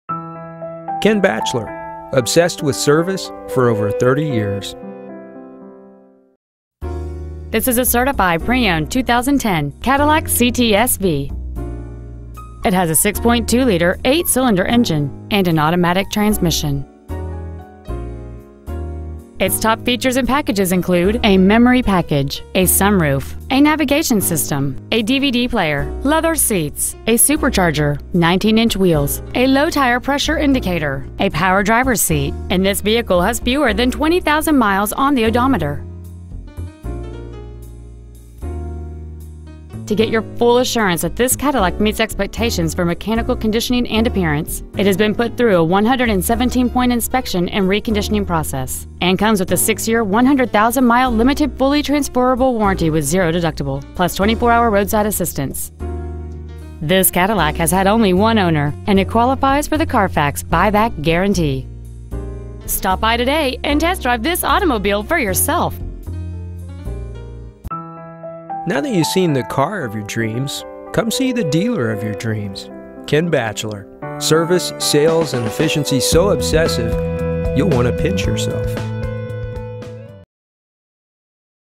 Certified 2010 Cadillac CTS-V San Antonio TX 78230
We are proud to present this 2010 Cadillac CTS-V .

 Year : 2010

 Make : Cadillac

 Model : CTS-V

 Engine : 6.2L supercharged V8

 Trans . : Automatic

 Exterior : Silver

 Miles : 19,964

 Interior : Black

 11001 IH-10 W

 Mission

 Values

 2010 Cadillac CTS-V San Antonio TX